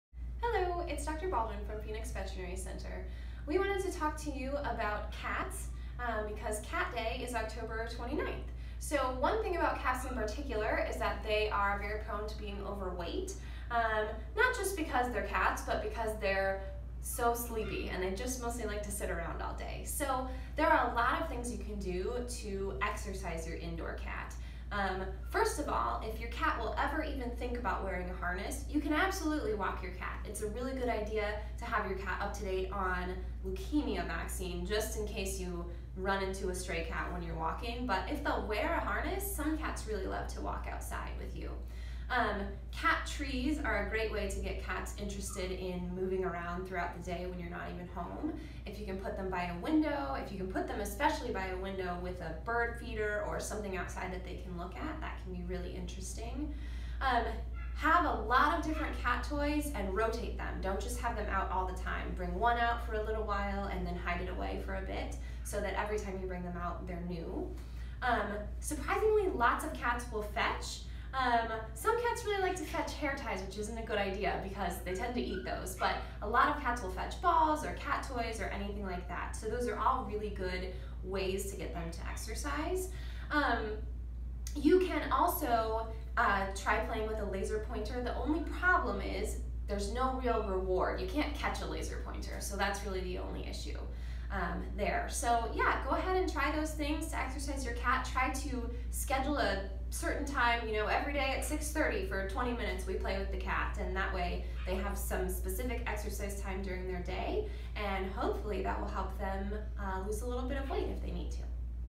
Exercising Your Cat
Cats are notoriously overweight which is why it is important to help them exercise, especially indoor cats. Here are some tips on how to exercise your cat.